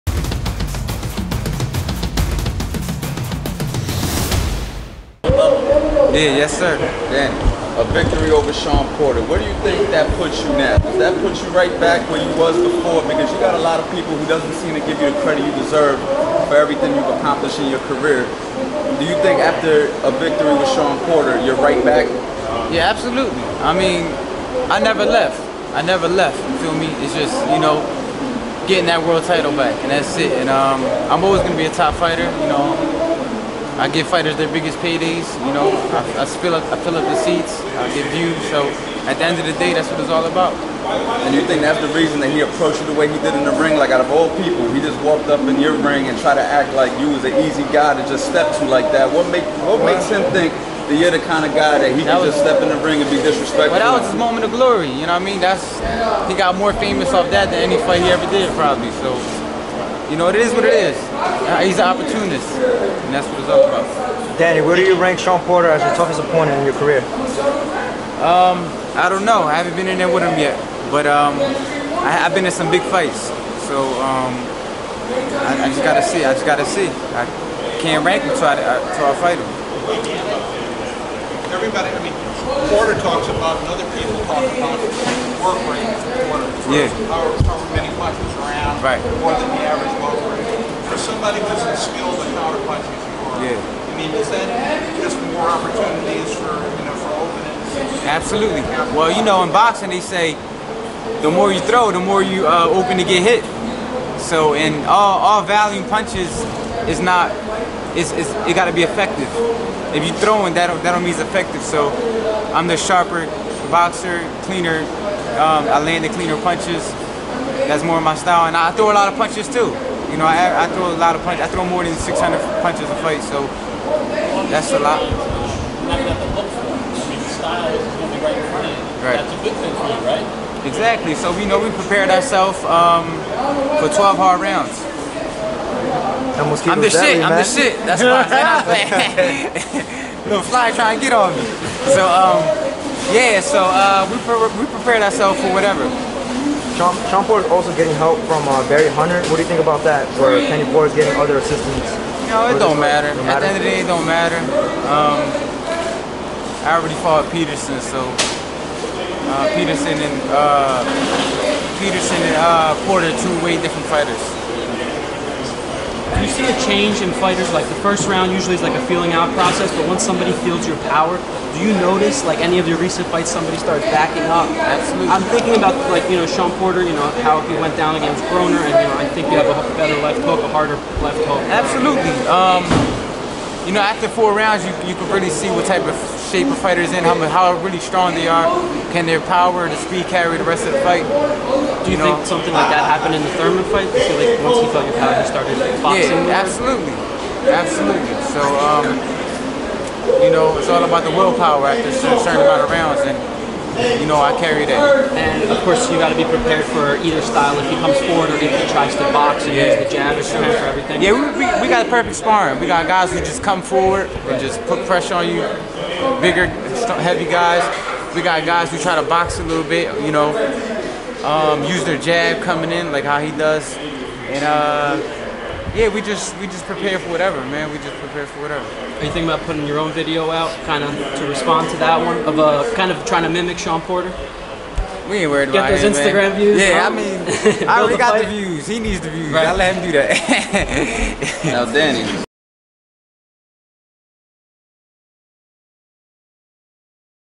DANNY GARCIA CALLS PORTER AN OPPORTUNIST "YOUR MOMENT OF GLORY WAS CALLING ME OUT!"
In this video, Danny Garcia goes off on Shawn Porter.